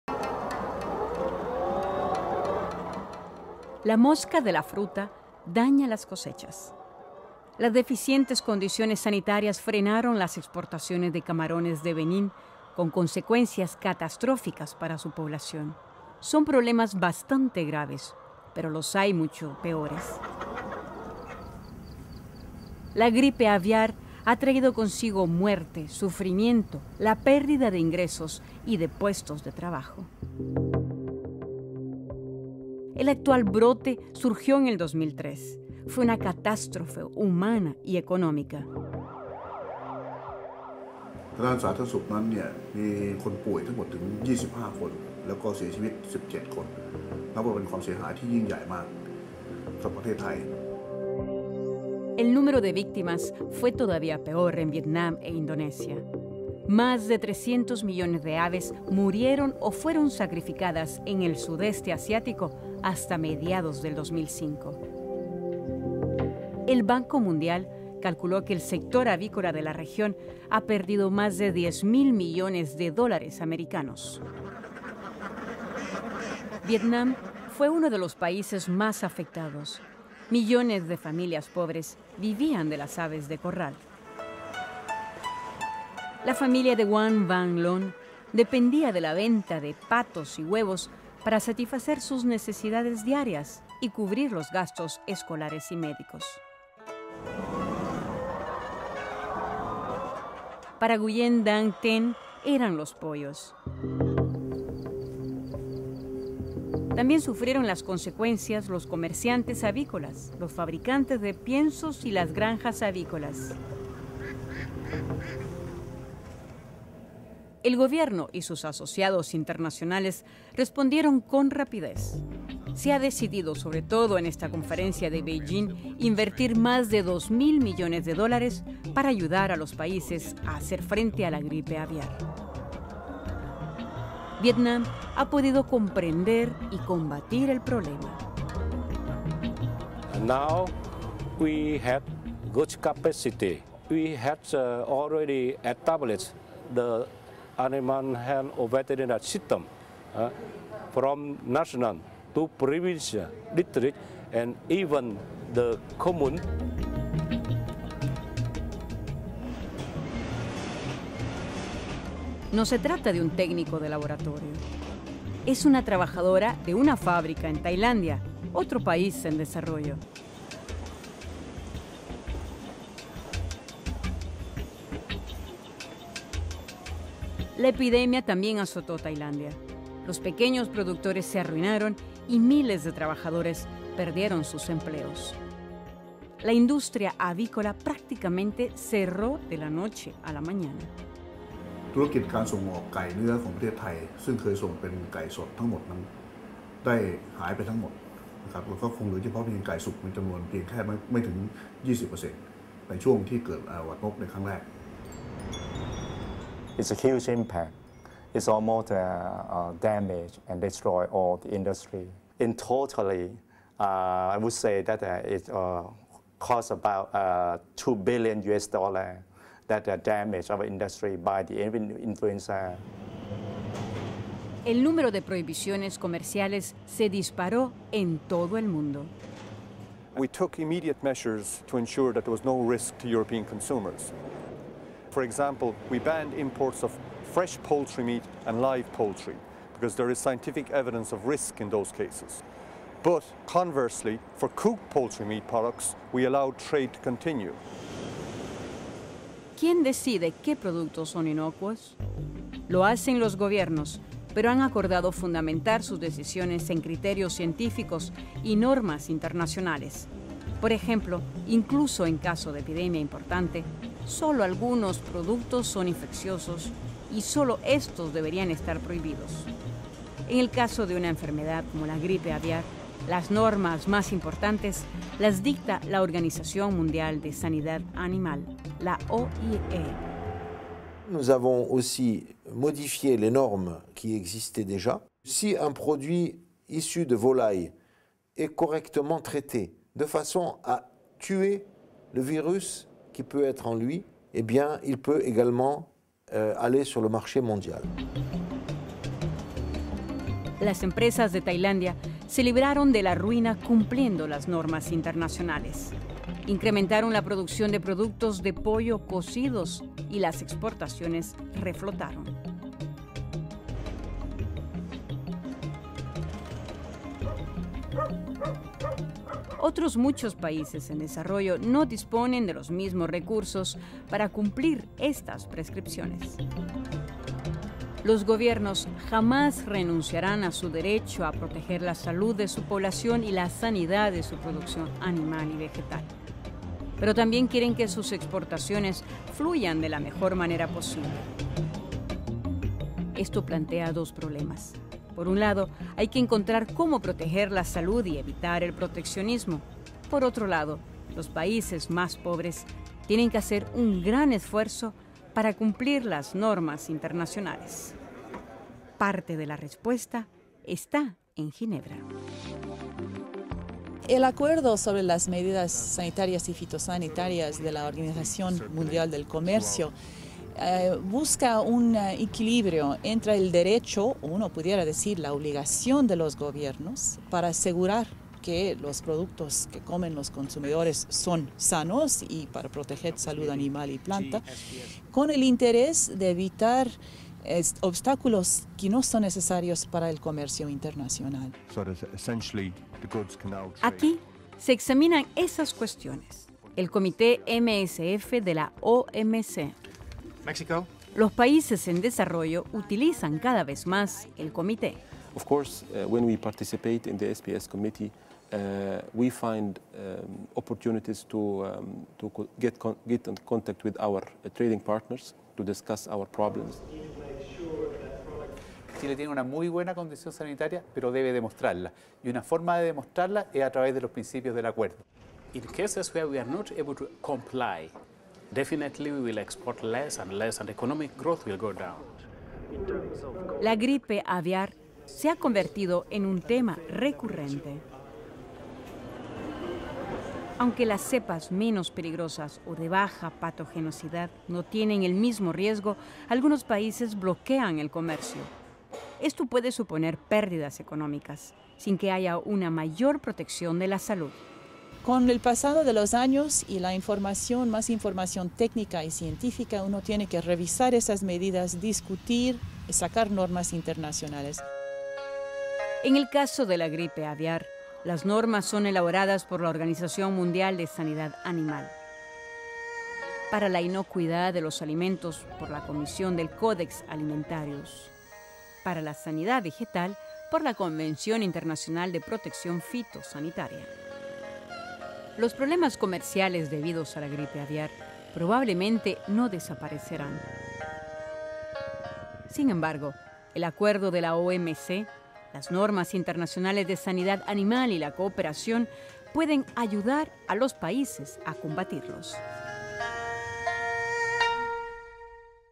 Comercio seguro - Sudeste Asiático
El comercio de productos alimenticios ayuda a aumentar los ingresos. Pero para beneficiarse plenamente, los países deben cumplir requisitos sanitarios y fitosanitarios. Reportajes en Tailandia y Viet Nam muestran como puede superarse el desafío.

Documental del Fondo para la Aplicación de Normas y el Fomento del Comercio (FANFC).